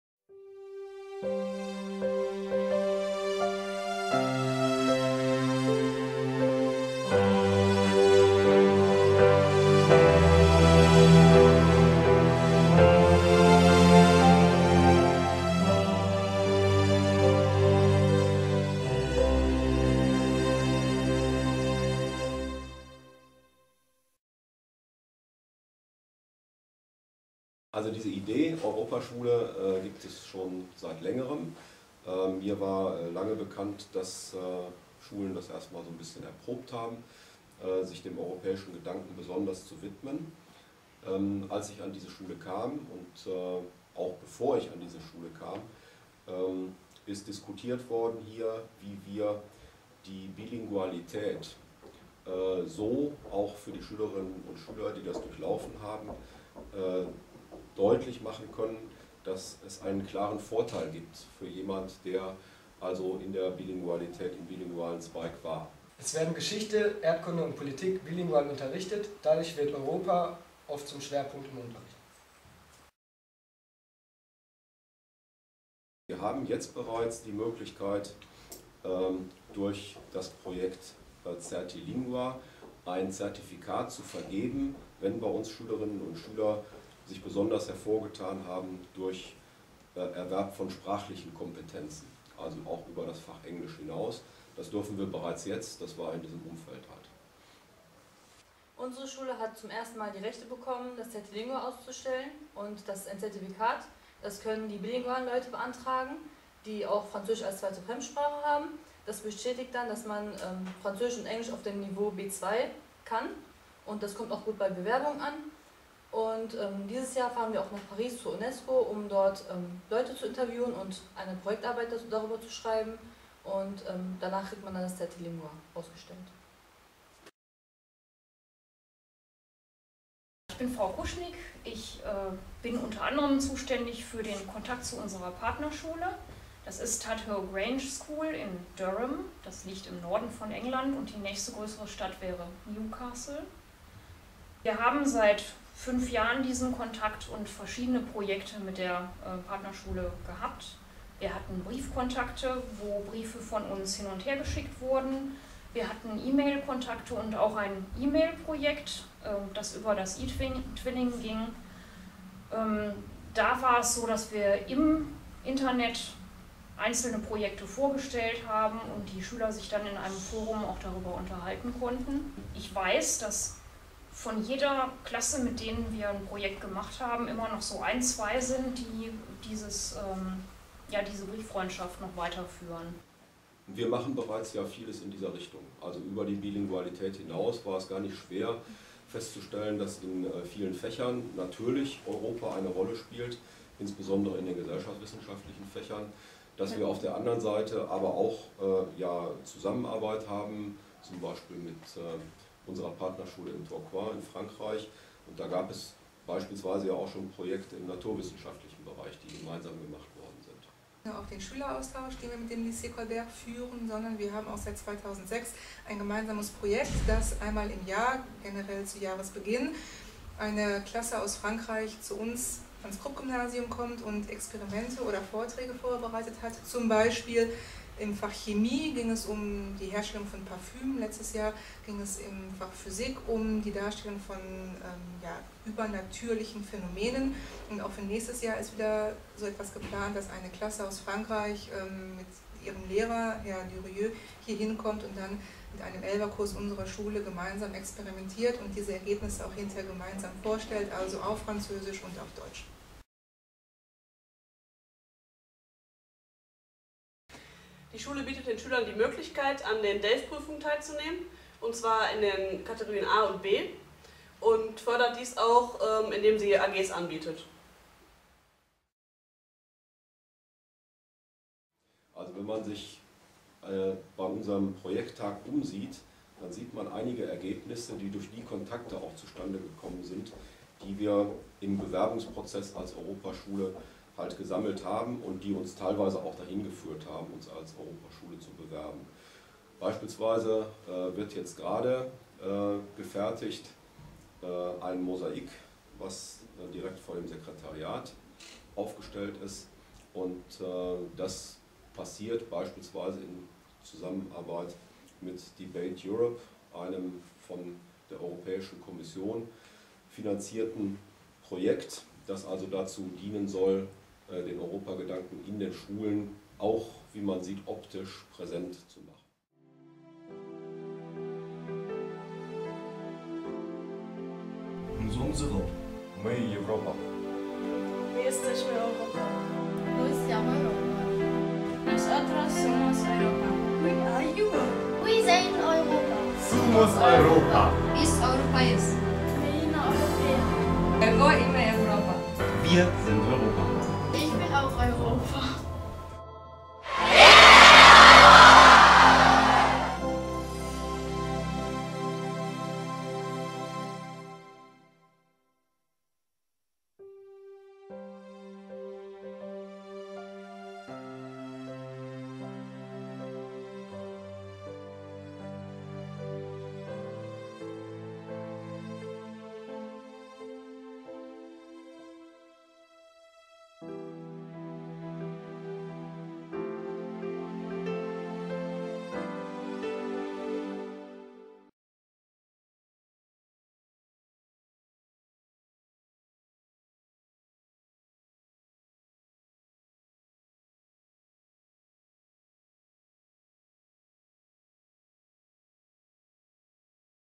Werbefilm "Krupp goes Europe"
Das Krupp-Gymnasium bewirbt sich zur Zeit um den Titel "Europaschule". Dafür muss es unter anderem nachweisen, dass es sich intensiv mit europäischen Inhalten auseinandersetzt. Deshalb haben wir einen Werbefilm während unseren Projekttagen "Krupp goes Europe" gedreht.
/// Regie & Musik: Alexander Kleinrensing /// Projektleitung: David Verhülsdonk & Barbara Rommerskirchen ///